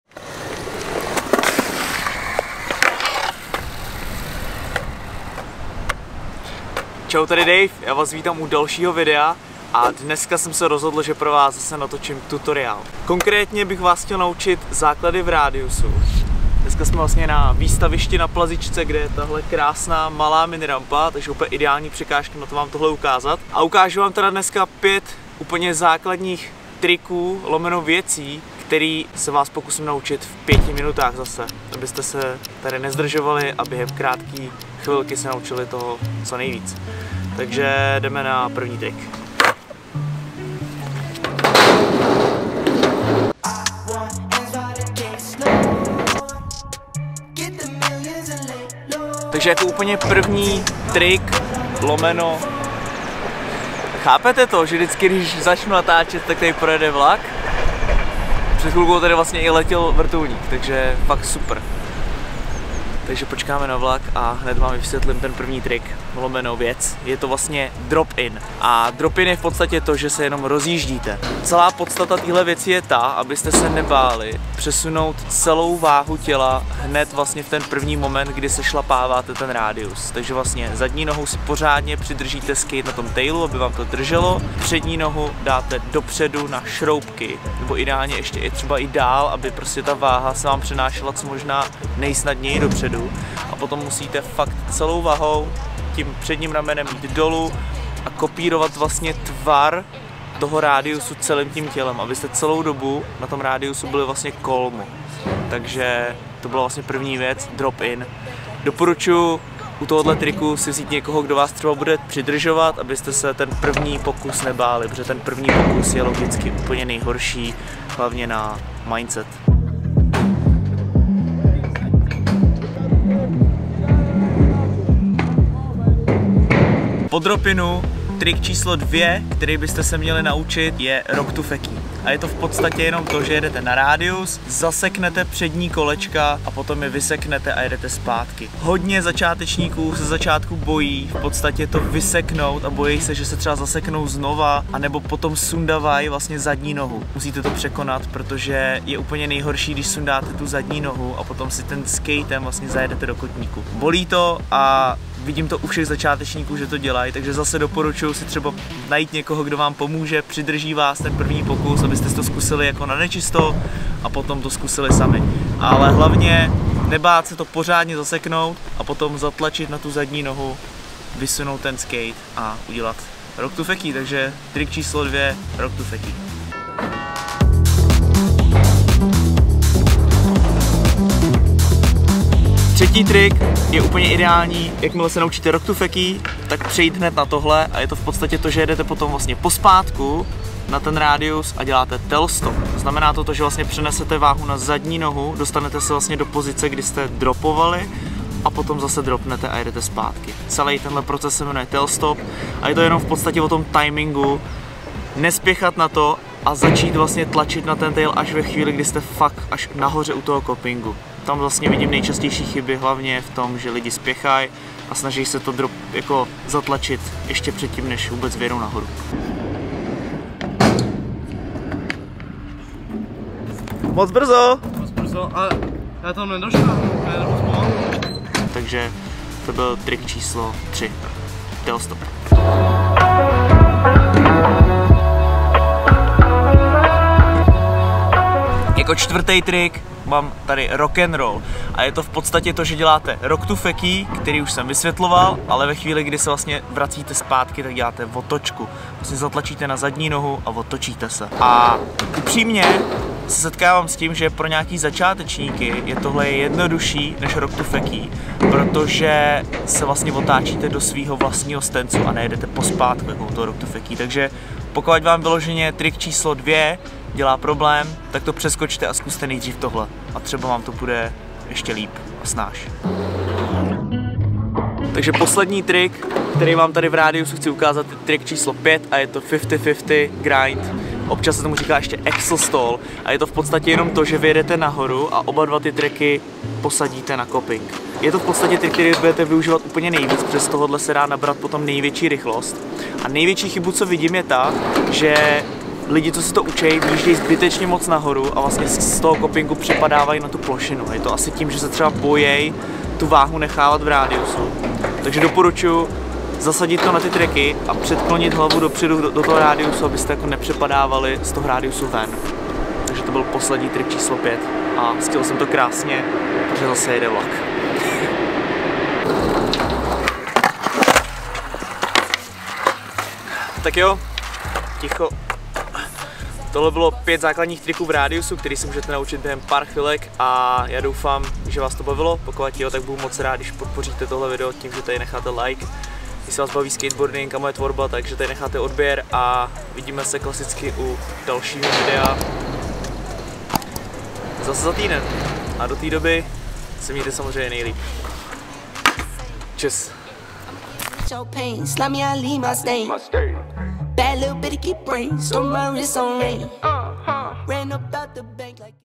5 ZÁKLADNÍCH TRICKŮ V RÁDIUSU ZA 5 MINUT!
Tentokrát se zaměříme na rádius a konkrétně jde o 5 základních věcí, kterýma doporučuji začít. 🛹